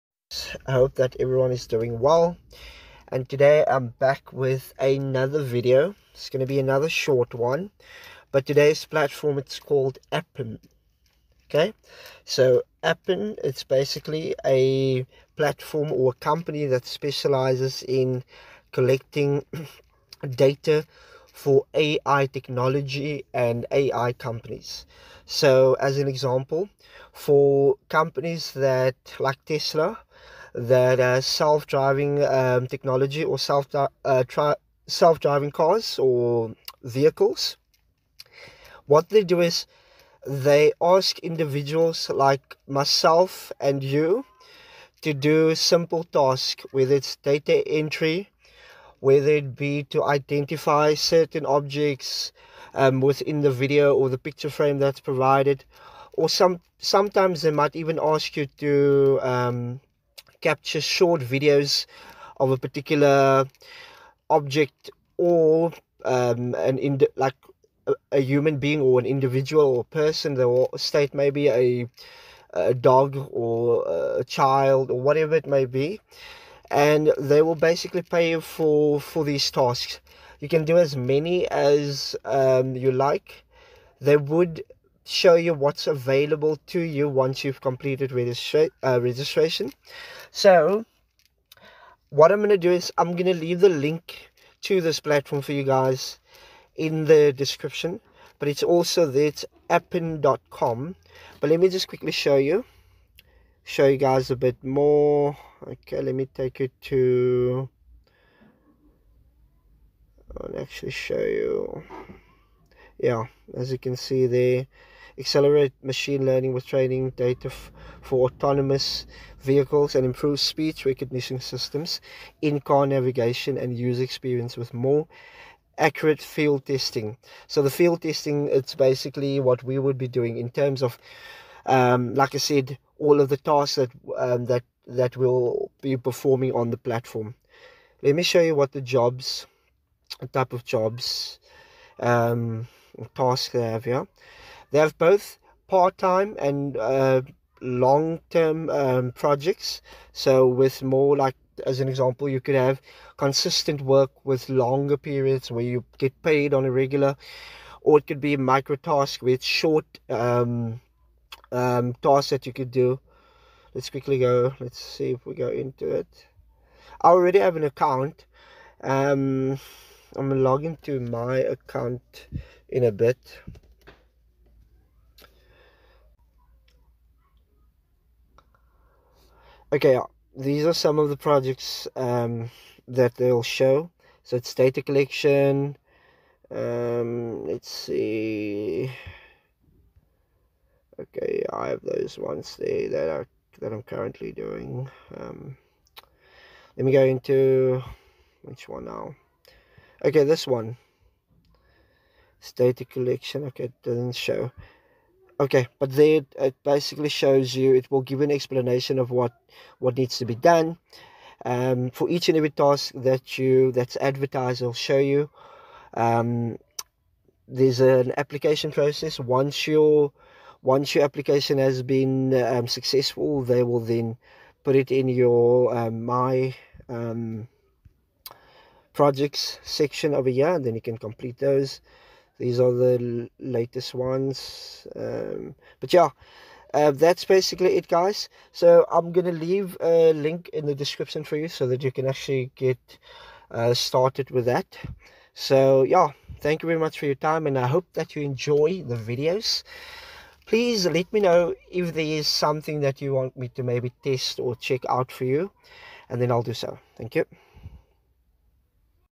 Make $100 A Day Doing AI Tasks With Appen - Make Money Online Today
Make money with a platform called appen..
"Appen"
"how to make money as a teen"
"how to make money as a beginner"